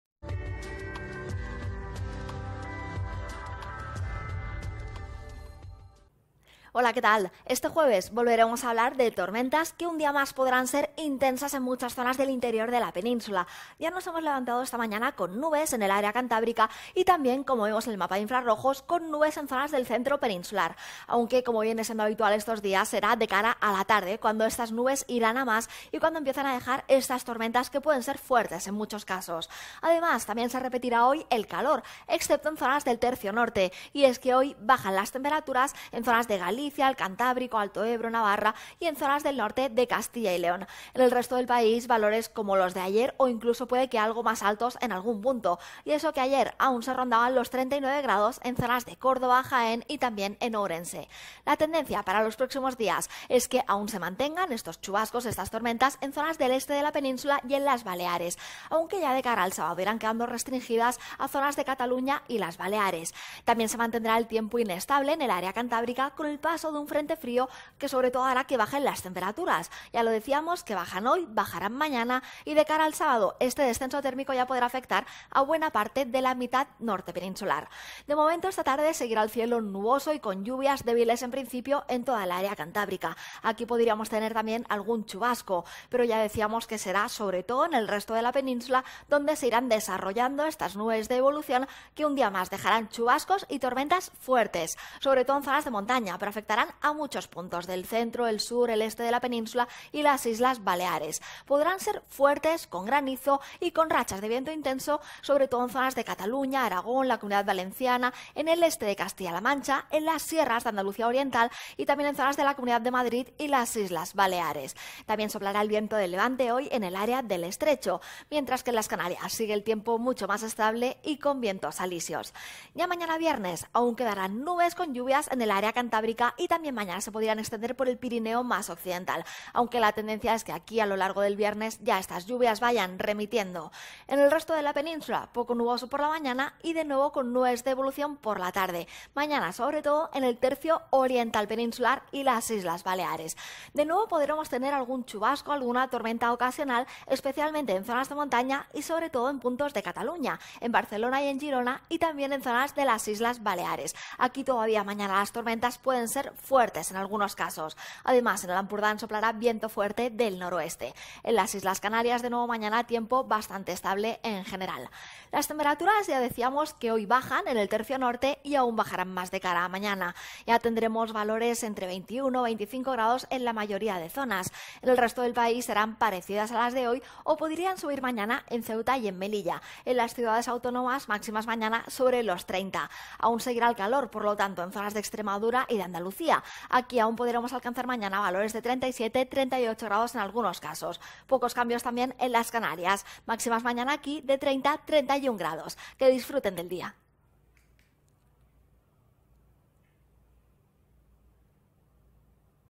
El Tiempo de AEMET 23-08-2018
Fenómenos significativos
Posibilidad de chubascos y tormentas puntualmente fuertes en el entorno de zonas montañosas del tercio este peninsular y en Baleares.

Predicción
En el área cantábrica, cielos nubosos con lluvias y chubascos. Predominio de cielos poco nubosos al principio en el resto del país, con nubosidad de evolución diurna en el interior que, por la tarde, dará lugar a chubascos y tormentas ocasionales en amplias zonas del este, sur y centro peninsulares y en Baleares; podrán ser puntualmente fuertes y con granizo principalmente en el entorno de sistemas montañosos del tercio oriental, norte de Aragón y Cataluña y en Baleares. Intervalos de nubes bajas matinales en Galicia, alto Ebro, área del Estrecho y norte de Canarias.
Temperaturas diurnas en descenso en Galicia, Cantábrico, Navarra, alto Ebro y norte de Castilla y León. Pocos cambios o en ligero ascenso en el resto.
Viento de levante en el área del Estrecho. Vientos de componente norte en el tercio norte peninsular; en Canarias, del noreste. Flojo en el resto, con rachas fuertes en zonas de tormentas.